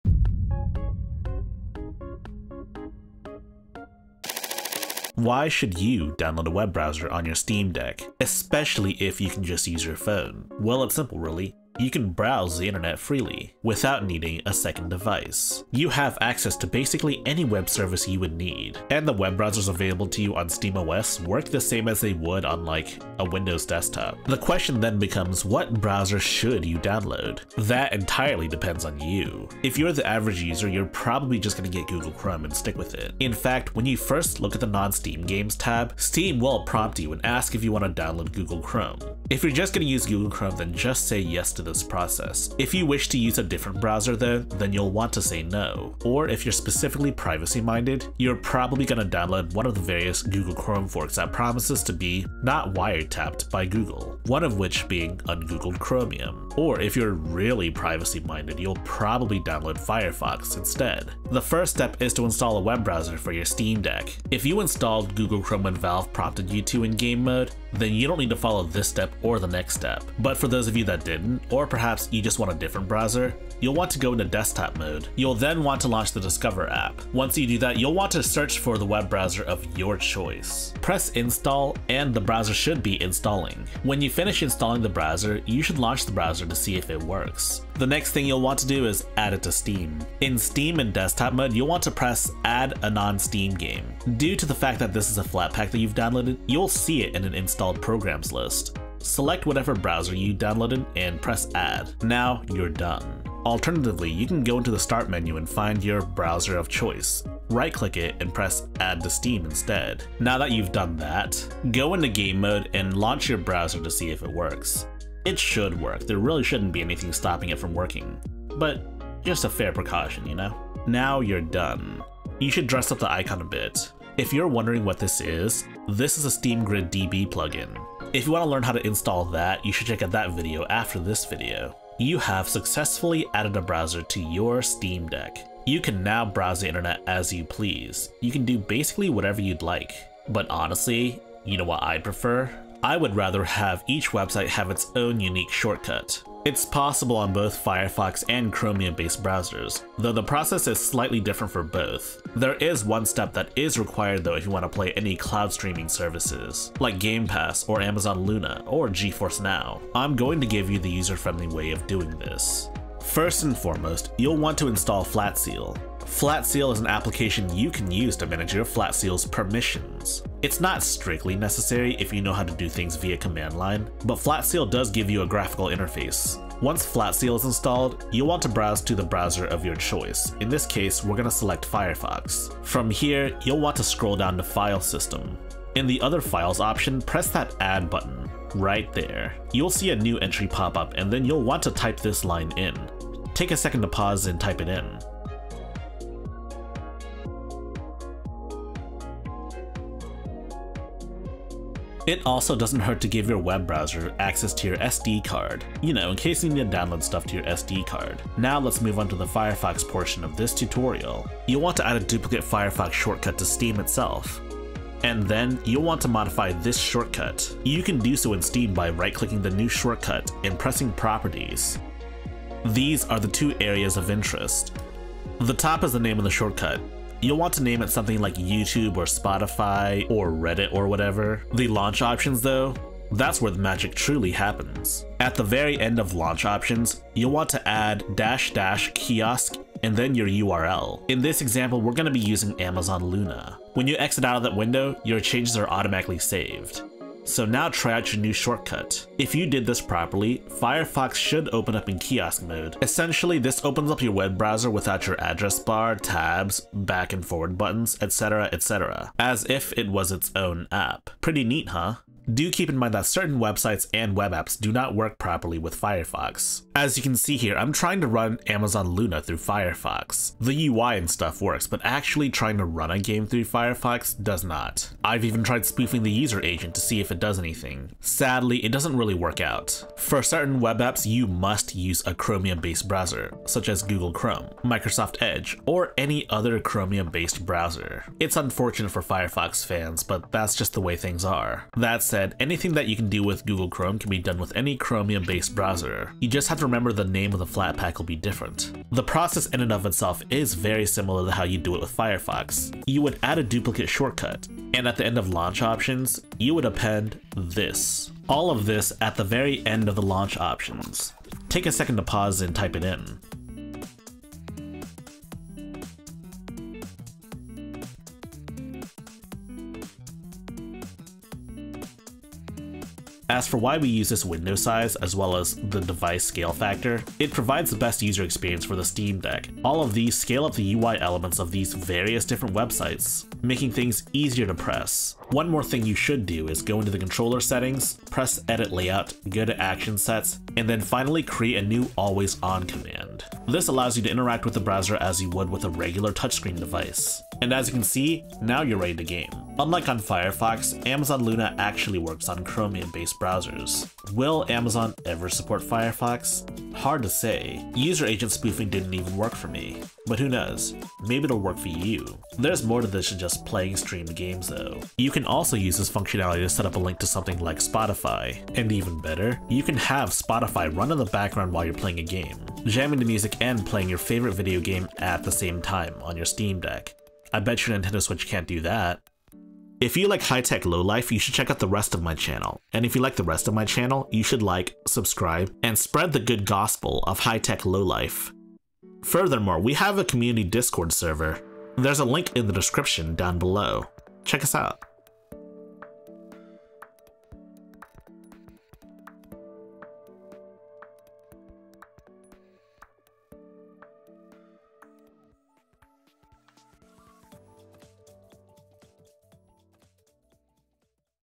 「How to get a Web Browser on Steam Deck」
Narrated by: Starlogical
Edited by: Starlogical

Append this to the end of your Launch Option (for Firefox)
--kiosk [INSERT URL HERE]

Append this to the end of your Launch Option (for Chrome)
--window-size=1024,640 --force-device-scale-factor=1.25 --device-scale-factor=1.25 --kiosk [INSERT URL HERE]